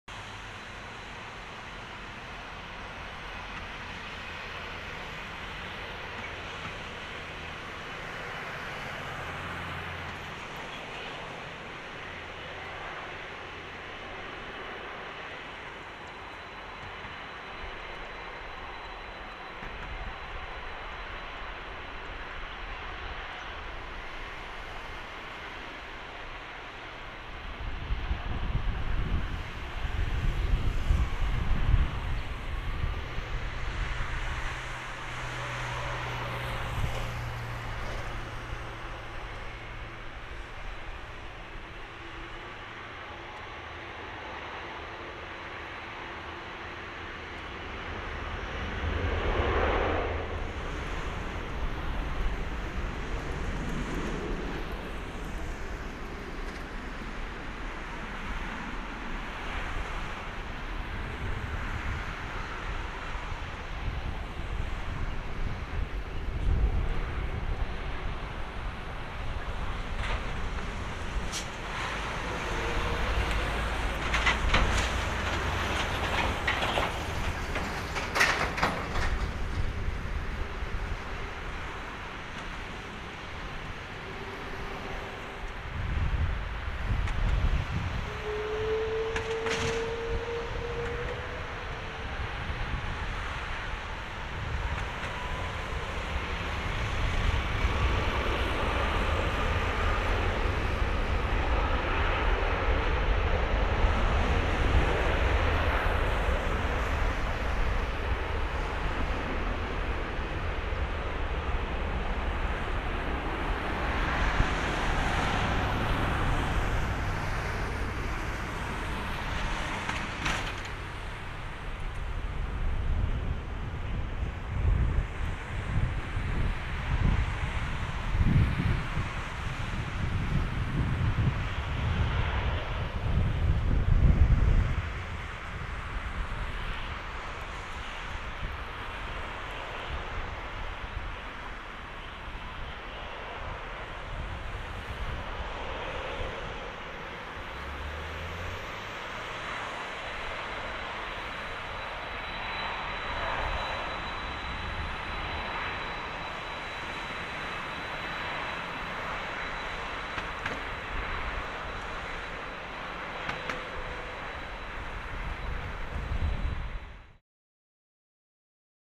Indonesia AirAsia X Airbus A330-300 PK-XRA Takeoff from NRT 16R
Indonesia AirAsia X Airbus A330-343X PK-XRA